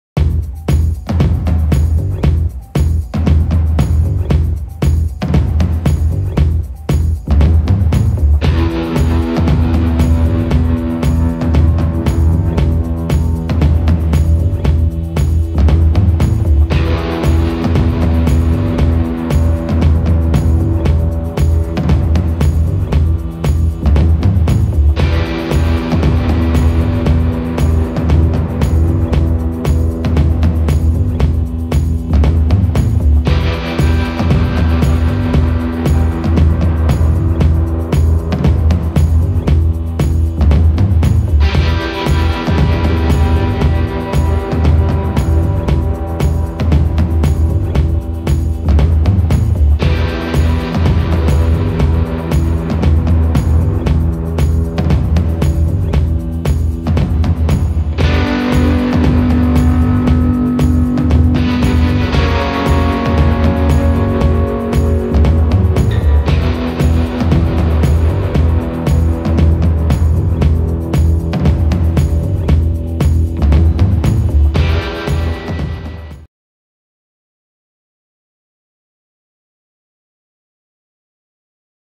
Drawing Meryl Streep
Half an hour of work in a short speed drawing video
Instagram.com/tomtrace